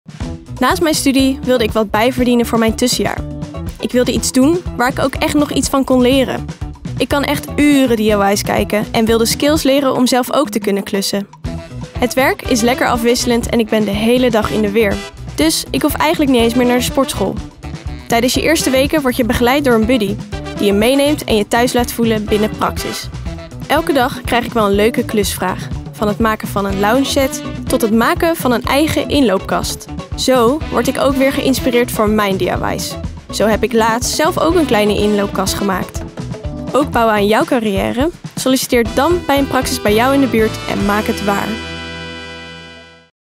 Een Bijbaan bij Praxis | VideoBird
Met dynamische en krachtig camerawerk legden we Danique vast op haar bijbaan bij Praxis.

Client: *Praxis*